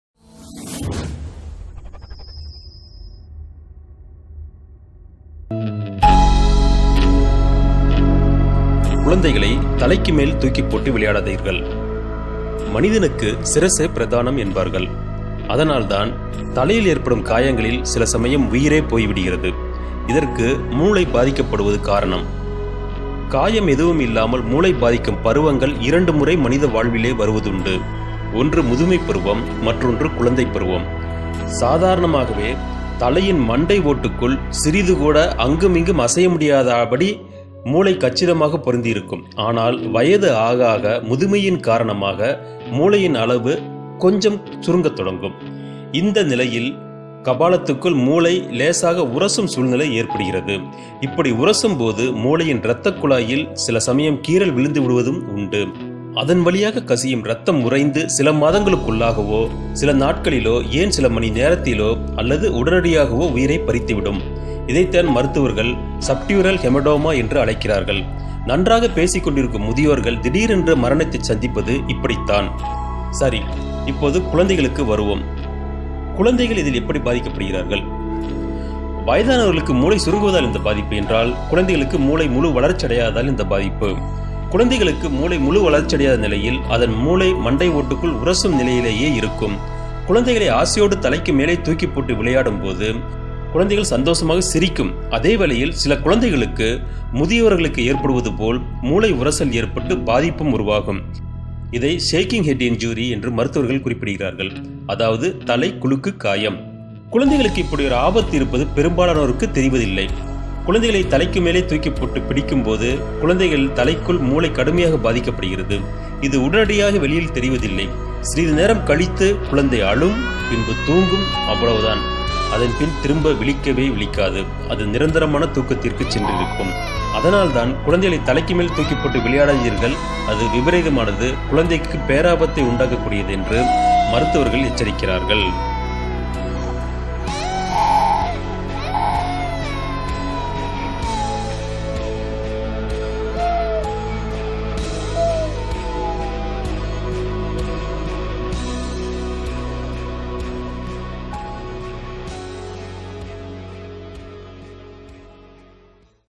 குழந்தைகள் உயிரைப் பறிக்கும் விளையாட்டு..!
குழந்தைகளை தலைக்கு மேலே தூக்கிப்போட்டு விளையாடுவது உயிருக்கு ஆபத்தாக முடியும் அபாயம் அதில் உண்டு. வெளிநாடுகளில் இப்படி குழந்தைகளை தூக்கிப்போட்டு விளையாடி அதனால் அந்த குழந்தை இறந்துபோக தந்தையை கைது செய்த சம்பவங்கள் எல்லாம் நடந்திருக்கிறது. எனவே குழந்தைகளிடம் விளையாடும்போது மிகவும் கவனம் தேவை. அதிலும் தலைக்கு மேலே தூக்கிப்போட்டு விளையாடாதீர்கள்.

ஓர் எச்சரிக்கை பதிவு!